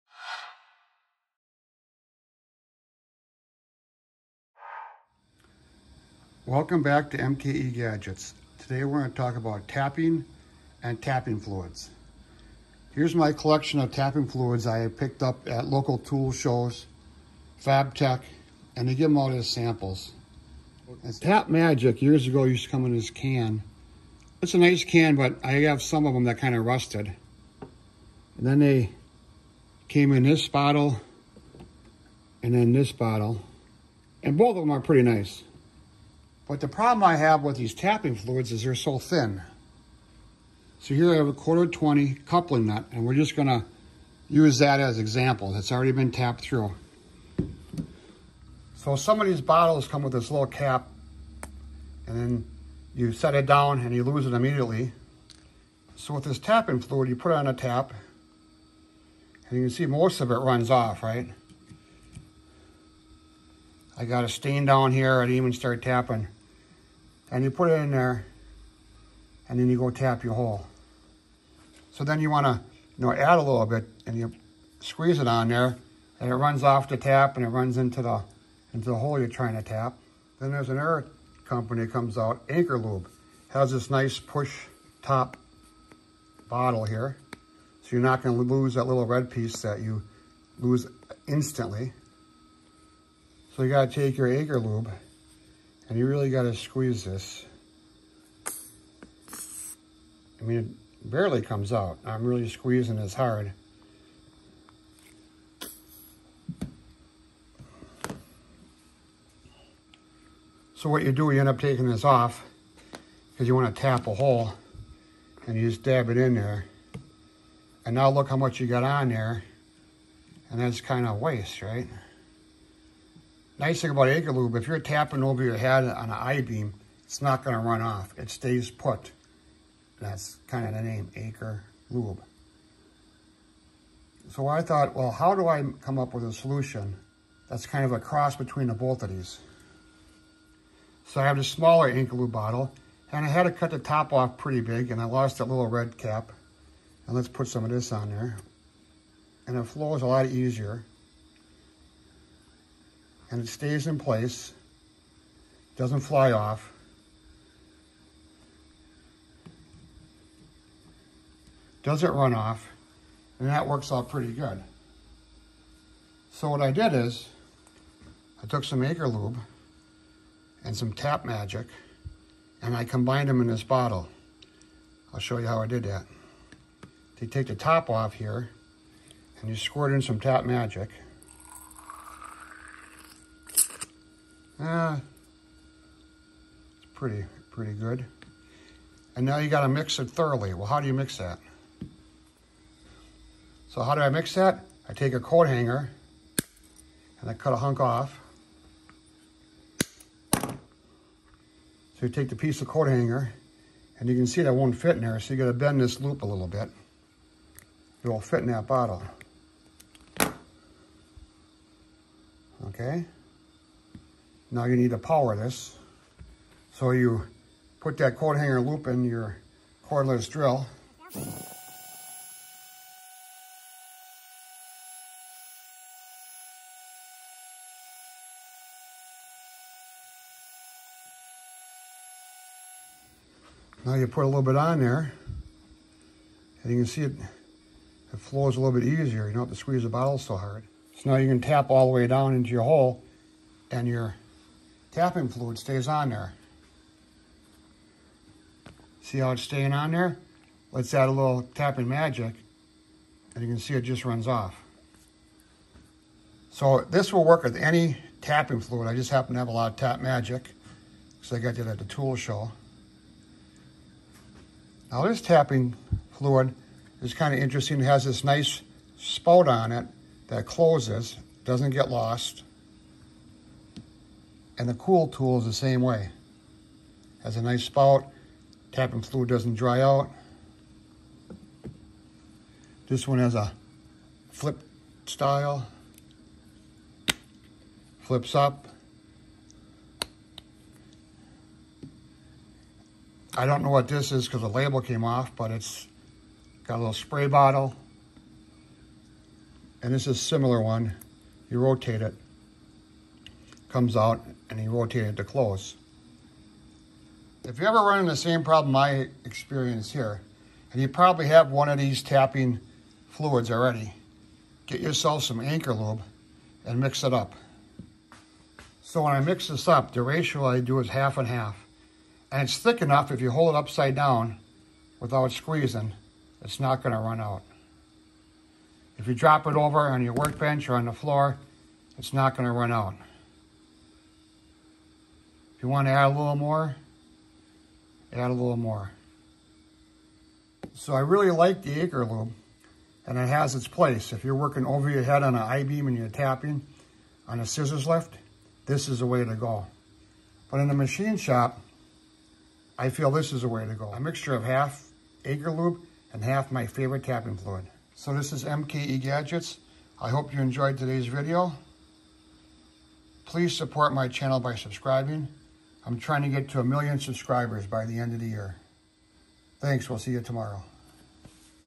Anchorlube Hack @ MKE Gadgets #221
In this video, I take two different tapping fluids, mix them together to make the ultimate tapping fluid.

Thanks see you tomorrow